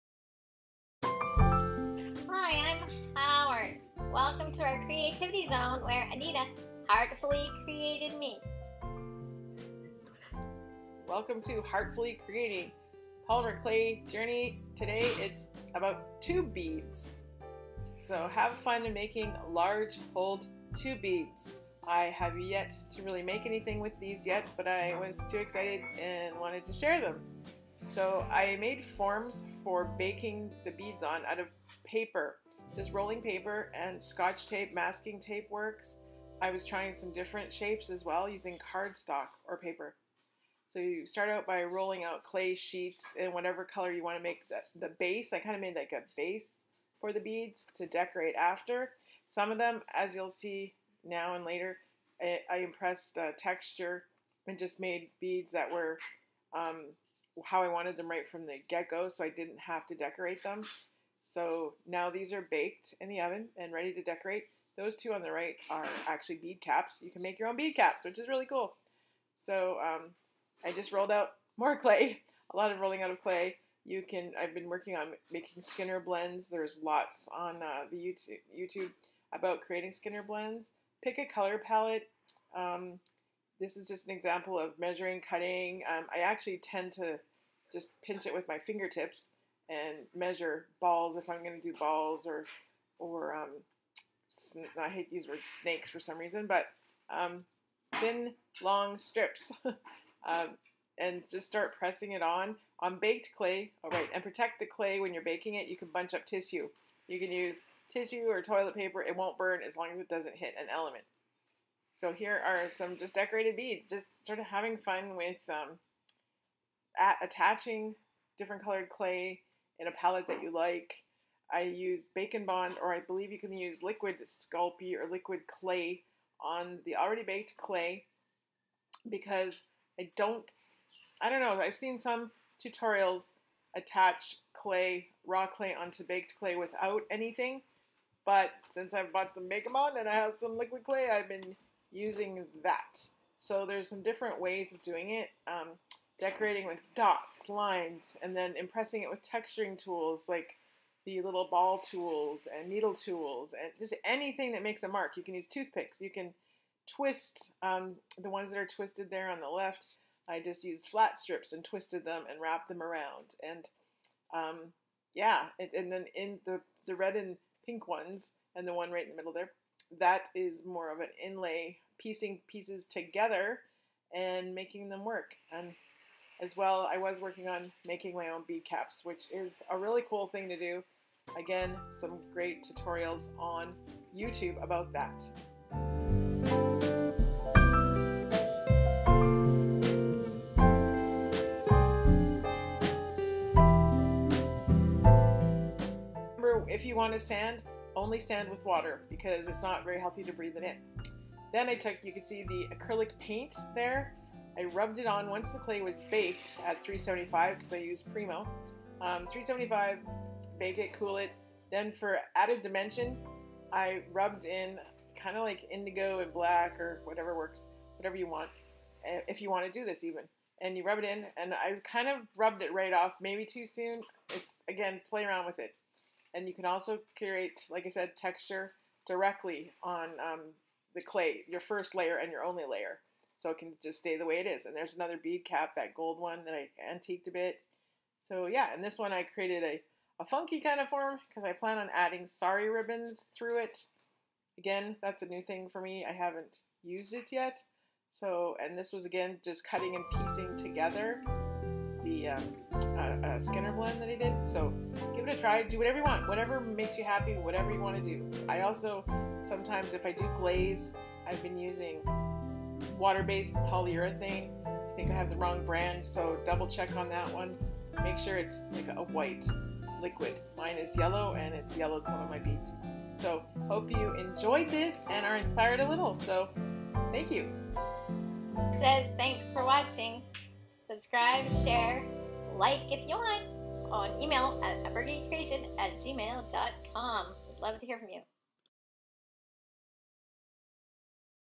Poylmer Clay Journey: Making Large Hole Tube Beads - DIY
HeArtfully Creating: Polymer Clay Journey. Making Large Holed Tube Beads.   Be inspired to try it!

This video supplements my HeArtfully Creating blog article.